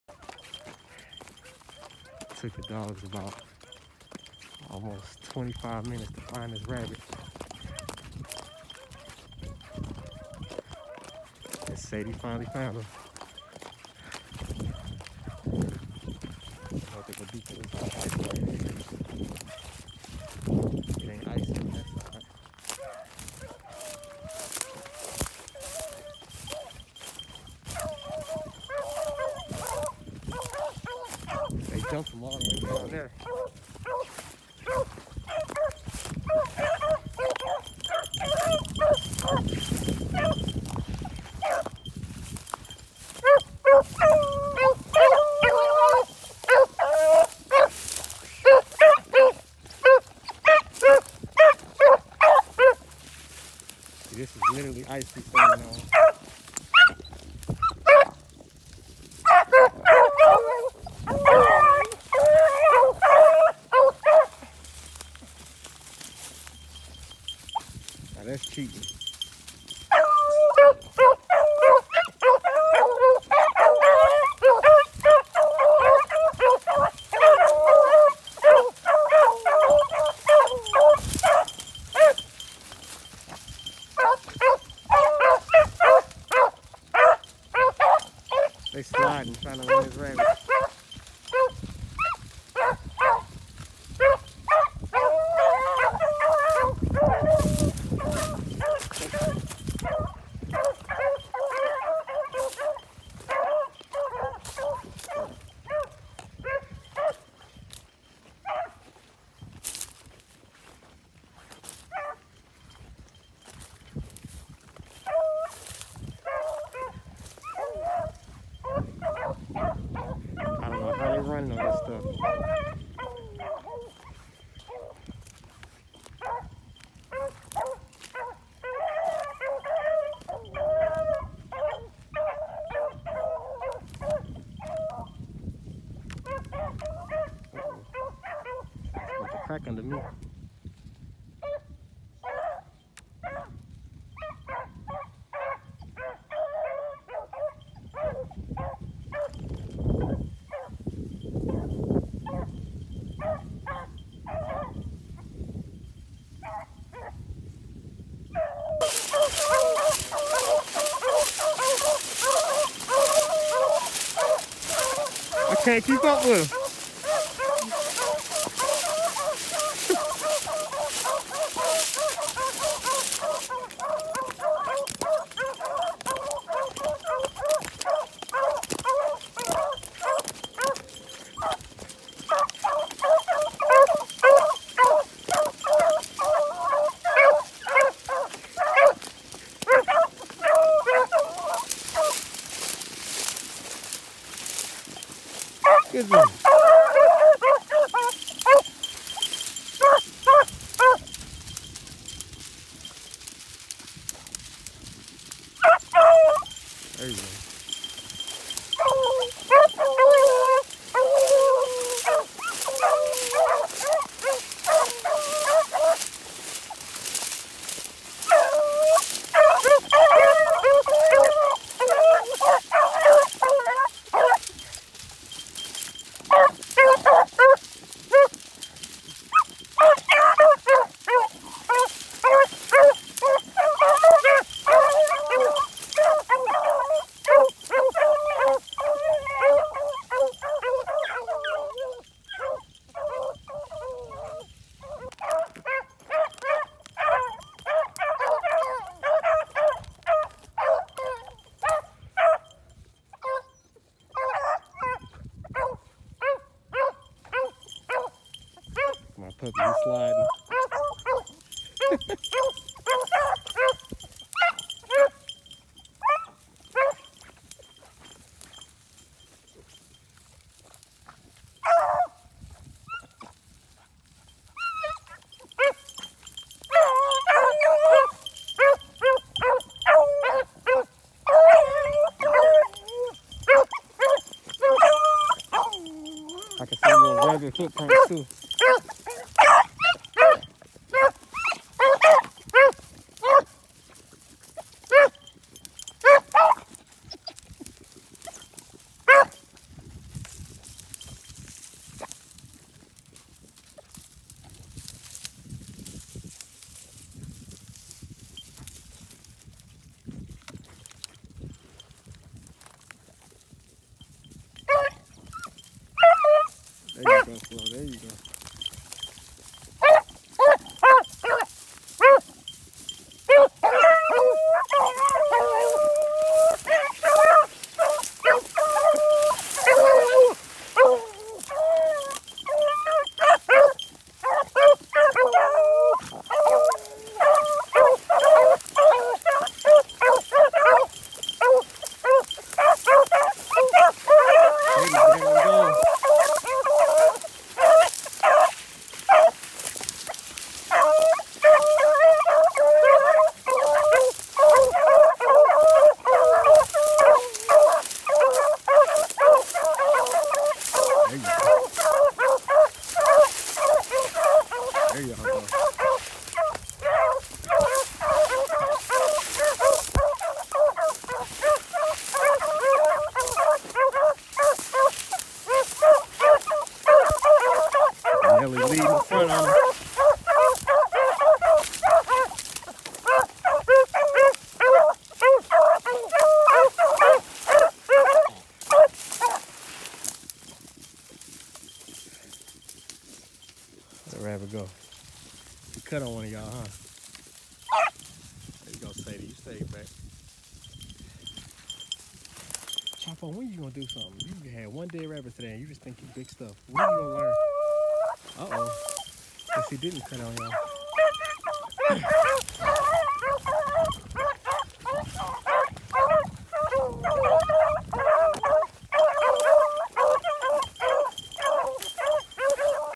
1-26-19 ice skating on bunnies
Dogs had to switch gears as This rabbit ran on solid ice but they did well enough for me